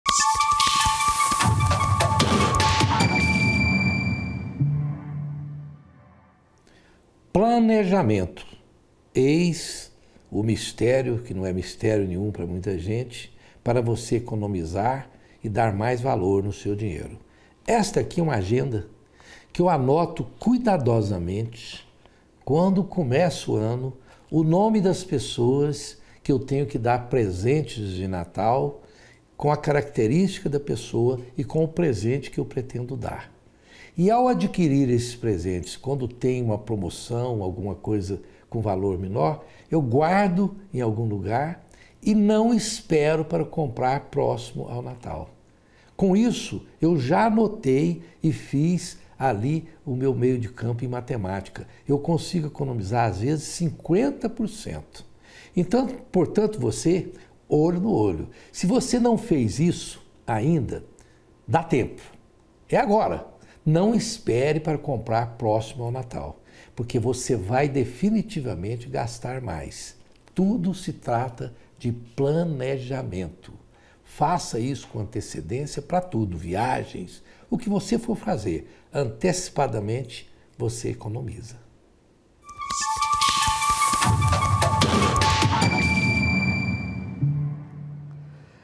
OLHO NO OLHO: PLANEJAMENTO REPRESENTA ECONOMIA
OLHO NO OLHO: HOJE ROSENWAL FERREIRA FALA SOBRE COMO PLANEJAMENTO REPRESENTA ECONOMIA NO NOSSO ORÇAMENTO.